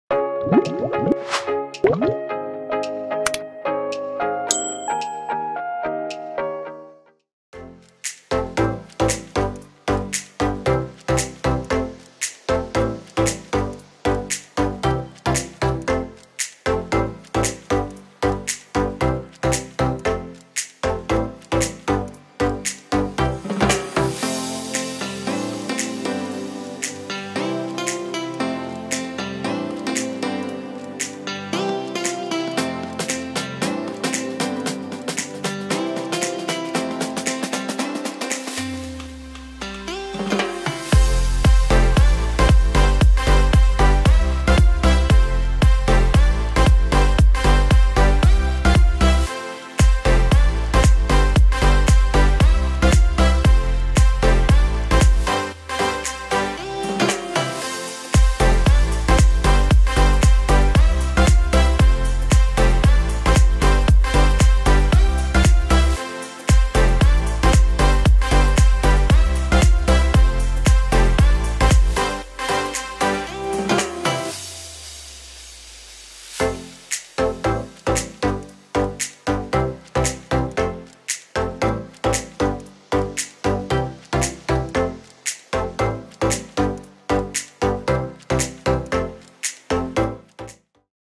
Easy Combination Tips For Office Wear | WomensByte
Complete video description - Every day the most difficult task after getting up is to decide “what to wear”. Most of us even get late because our major portion of time gets consumed in deciding what should we wear to the office so that we don’t look awful. So to ease your tension we have some easy combination tips for you.